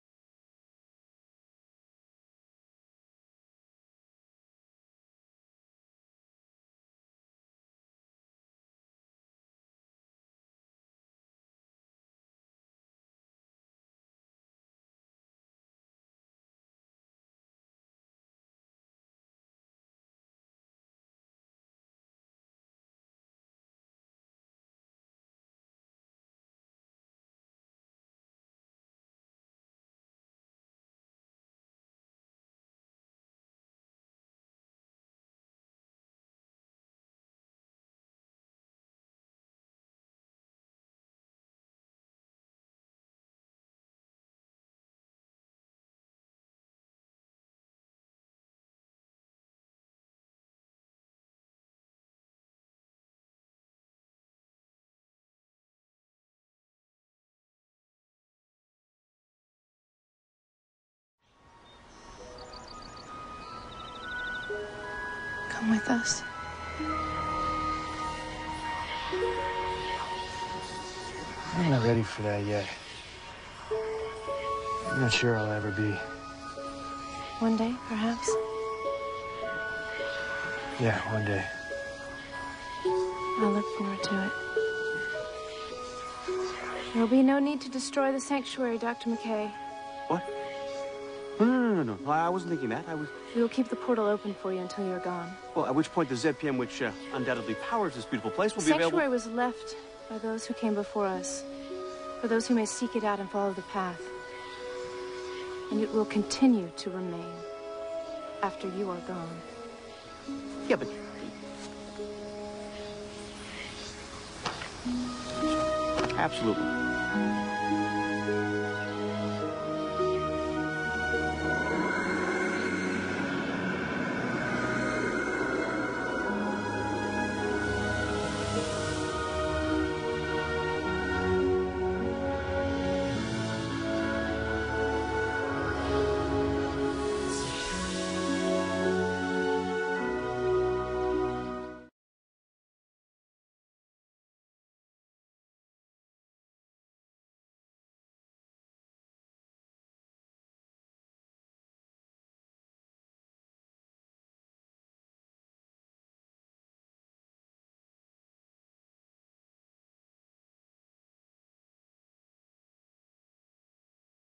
PŘEVIBROVÁNÍ CZ/EN
Převibrování fyzického těla člověka ze 3. do 5. dimenze po splnění světelného poslání.
Živé lidské tělo zvyšuje frekvenci kmitání - vibraci atomů díky zvyšování láskyplnosti a tak se může definitivně přemístit do 5. dimenze planety Země